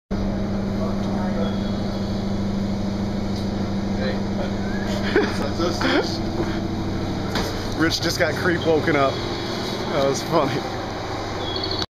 JM Adventure: Rich creep train
richi made the unfortunate mistake of falling asleep on the train... so he recieves meibatsu!